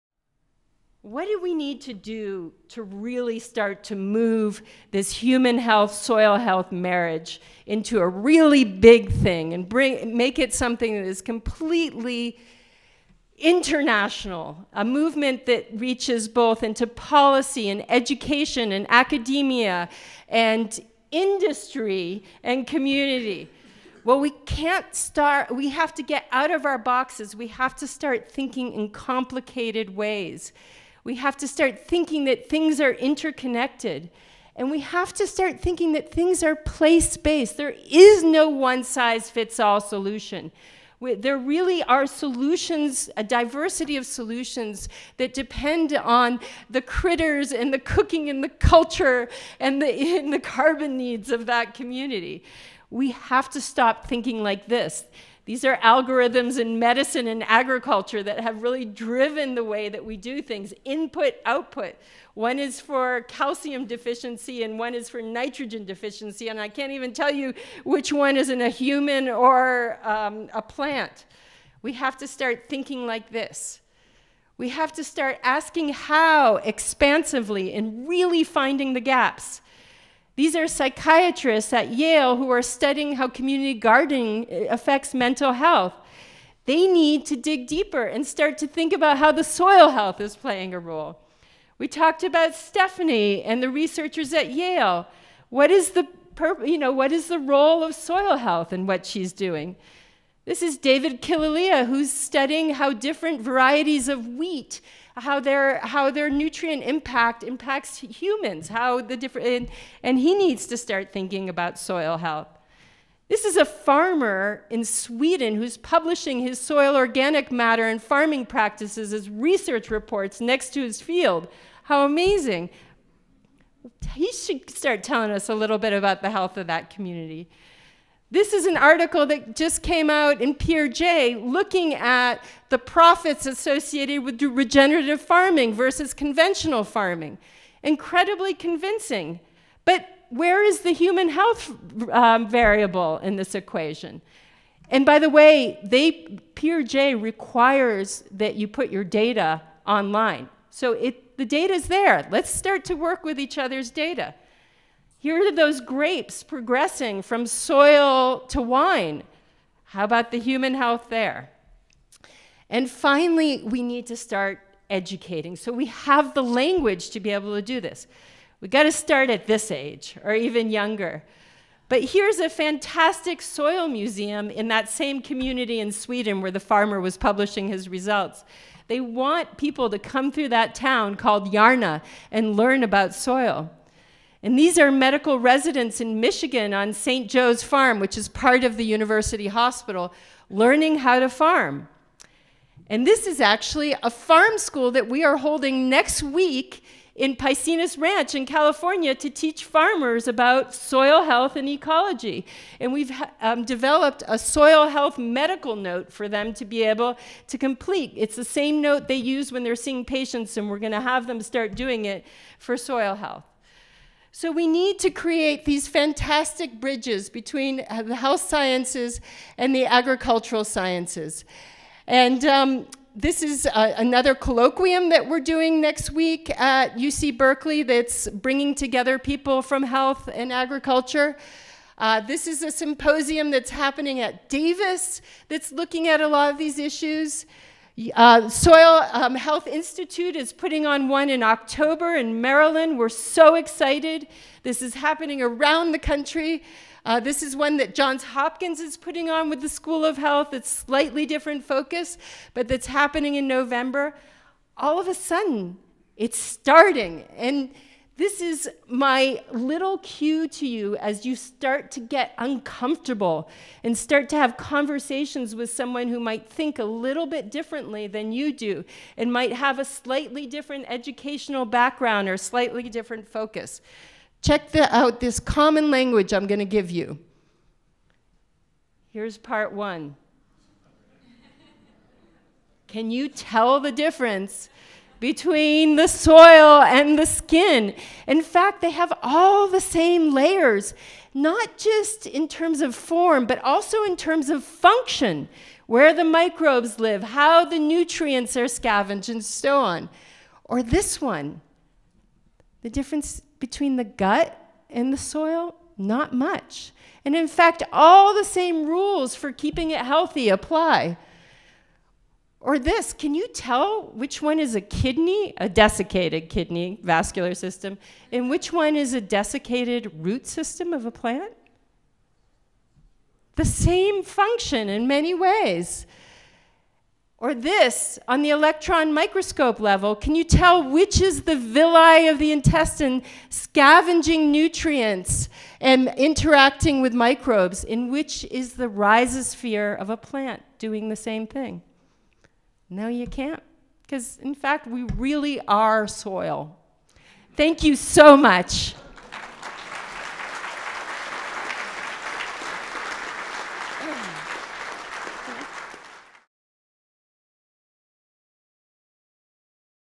Our Soil, Ourselves: The Links Between Soil and Human Health - Daphne Miller
Dr. Daphne Miller connected soil health and human health during SHI’s 3rd Annual Meeting. Dr. Miller is an Associate Clinical Professor at the University of California Berkeley who has focused her work during the past 15 years on aligning agriculture and conservation with human health.

Dr. Miller organized a condensed set of four major connections that link soil health to human health: cooking, critters, carbon, and community. Through the lens of these four interlocking connections, Dr. Miller described examples of how understanding and caring for the health and diversity of agricultural ecosystems are vital to the greater health of the human population.